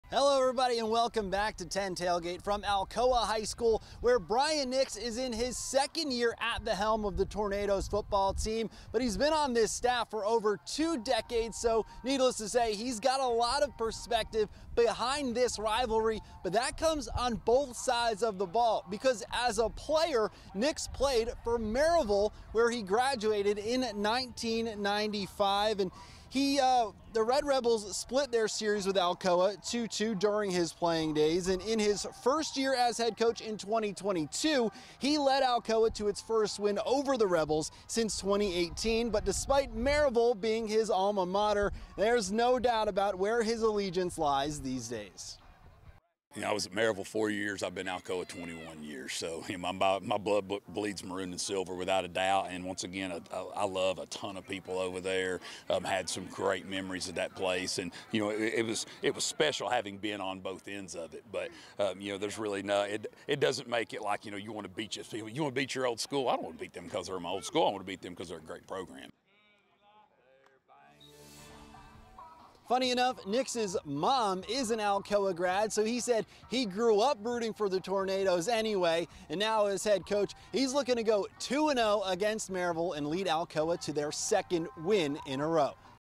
Alcoa head coach has been on both sides of rivalry
Alcoa head coach Brian Nix is only his second year at the helm, but he's been on the Tornadoes staff for more than two decades.